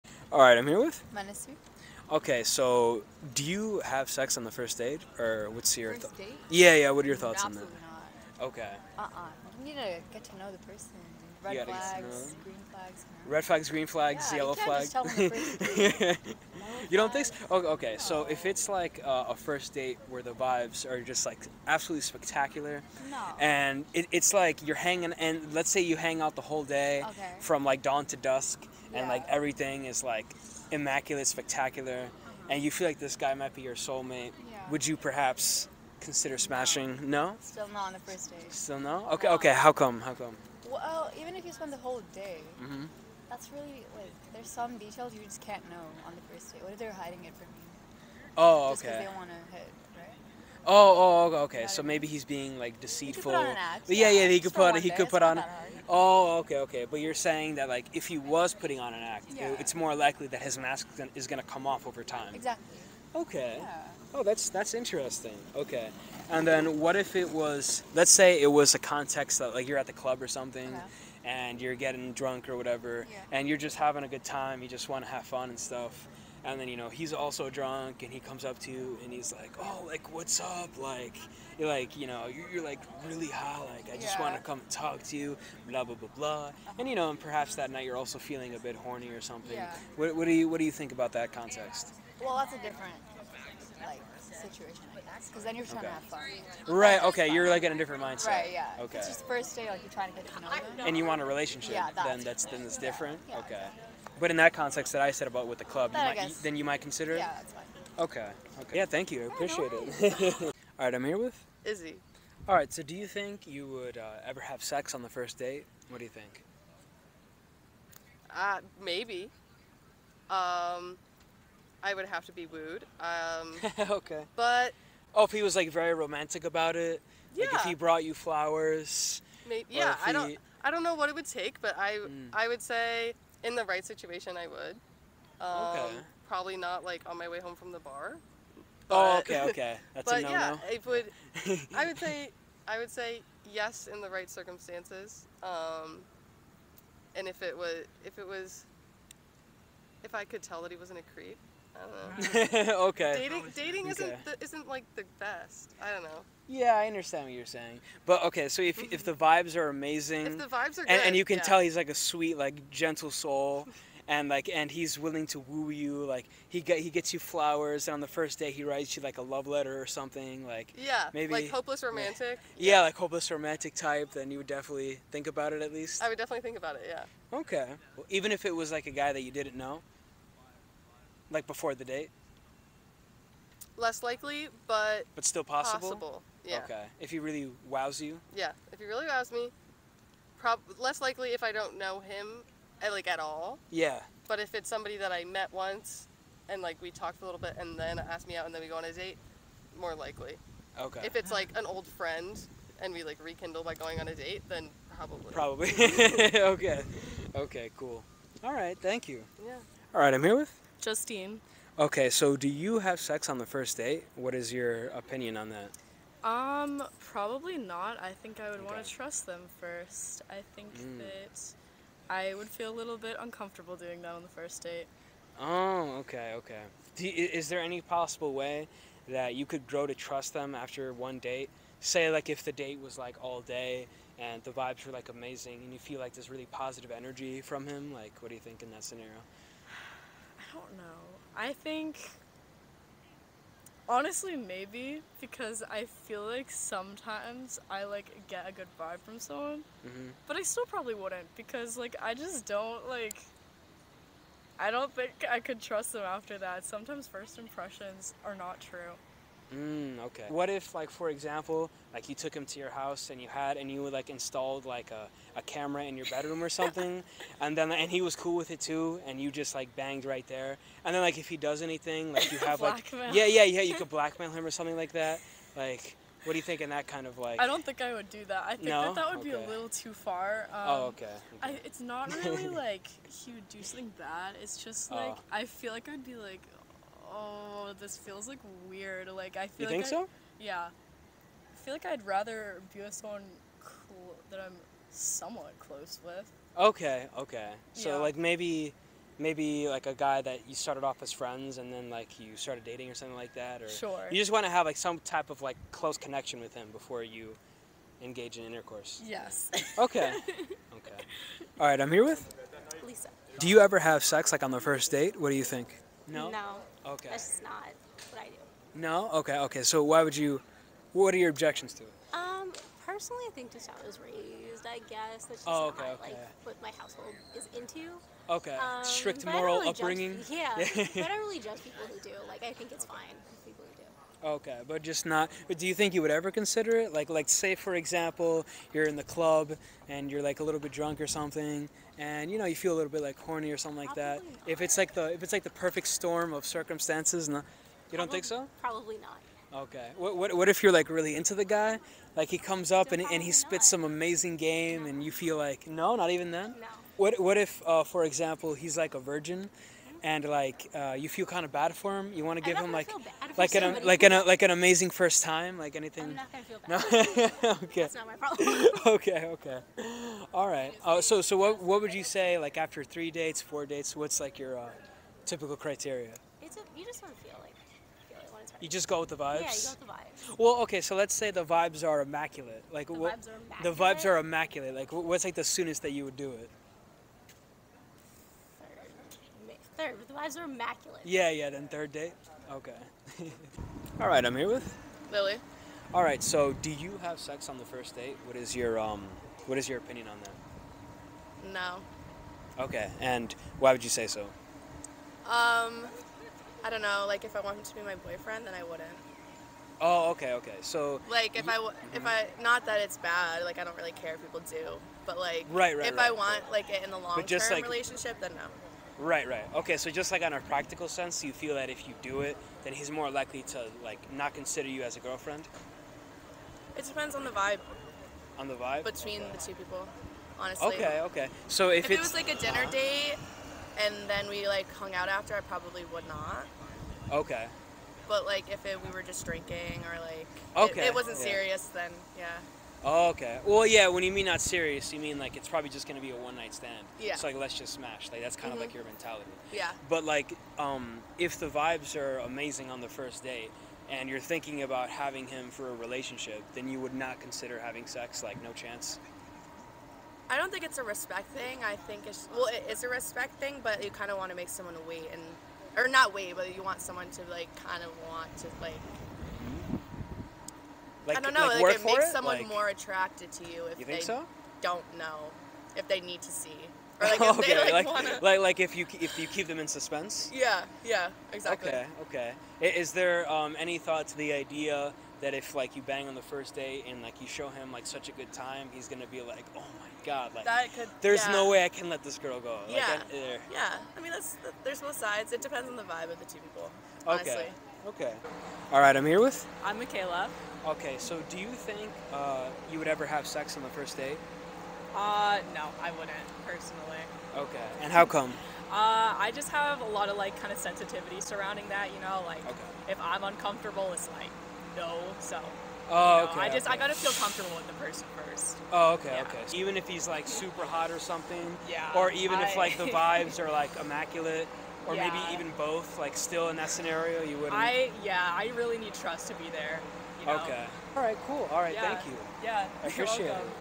Would you bang on the first date? | Public Interview
For this video, I asked girls "would you bang on the first date?" How do girls feel about smashing right away? Would they consider it if the vibes were amazing? Would they smash if the guy was really hot? Does it depend on their mood?